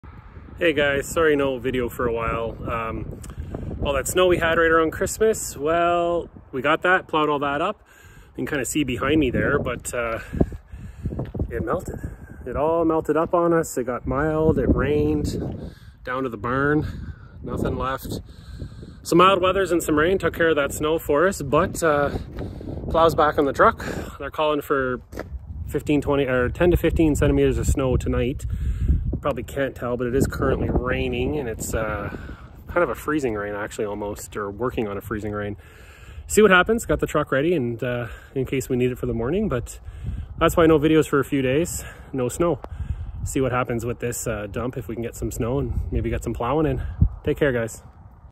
Snow update!
Jus thought I would put something up as I haven't for a few days now, as we lost most of our snow.
Hope everyone had a great Holiday and welcome to the New Year.